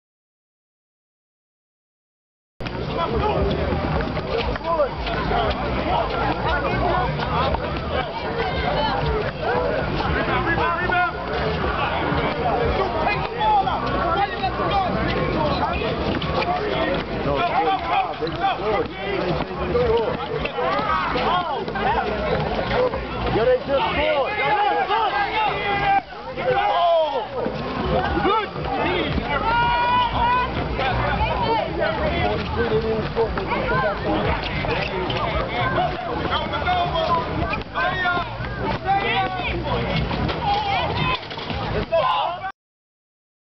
Adidas Sebastian Telfair Tournament in Coney Island July2007
Sebastian Telfair and Adidas sponsored a local high school tournament in Coney Island.